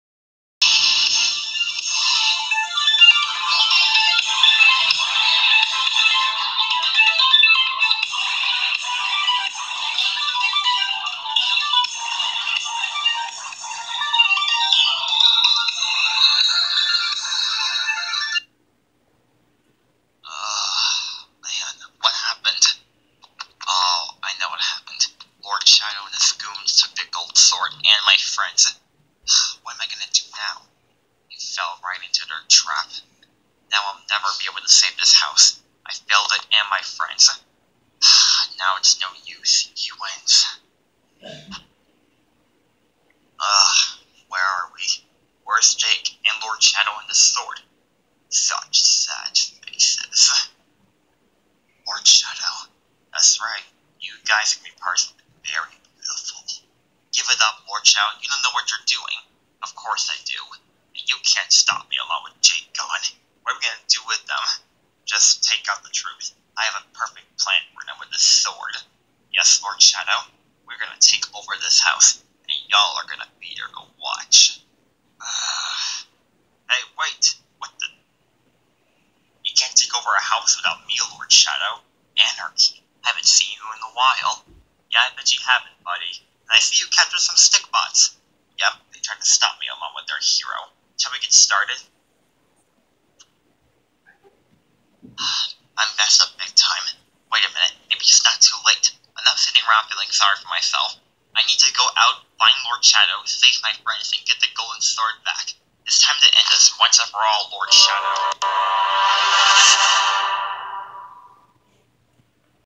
The Rise of Stikbots Season 3 episode 1 (season premiere)
It's back for a final season, a new villain name Anarky has appeared and wants to be with Lord Shadow while he takes over the house.